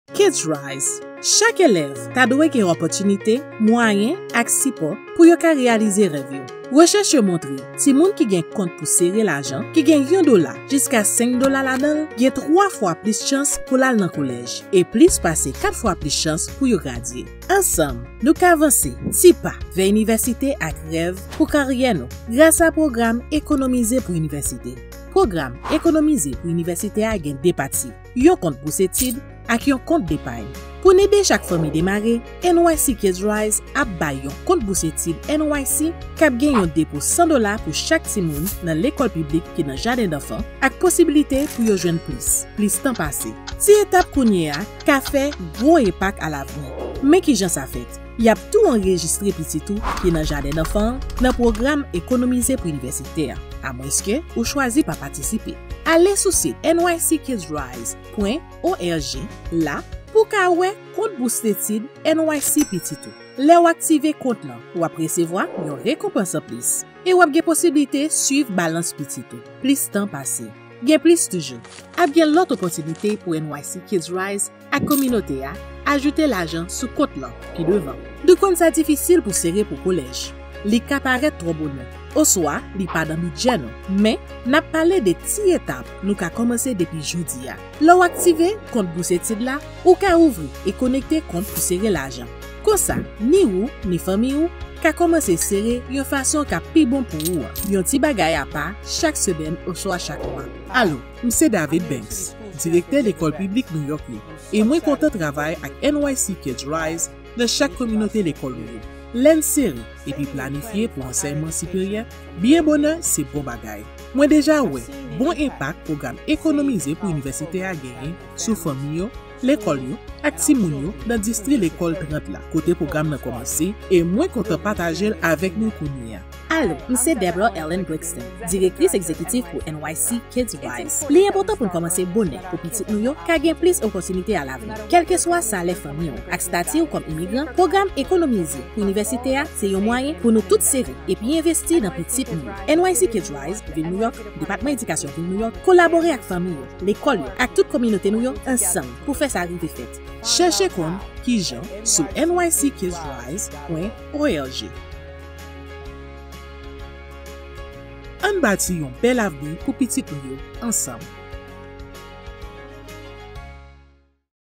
Byenveni nan Pwogram Ekonomize pou Inivèsite NYC Kids RISE la!
Nan ane akademik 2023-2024, elèv ki nan klas jadendanfan, nan klas premyè ane ak nan klas dezyèm ane nan lekòl piblik NYC (ki enkli lekòl piblik endepandan [charter school] k ap patisipe yo) ka antre nan Pwogram nan, kèlkeswa revni oswa estati imigrasyon fanmi yo.

Timoun ki elijib yo enskri otomatikman epi yo resewva yon Kont Bousdetid NYC (NYC Scholarship Account) gratis, ki gen yon premye depo $100 sou li sof si paran yo oswa responsab legal yo chwazi pa patisipe.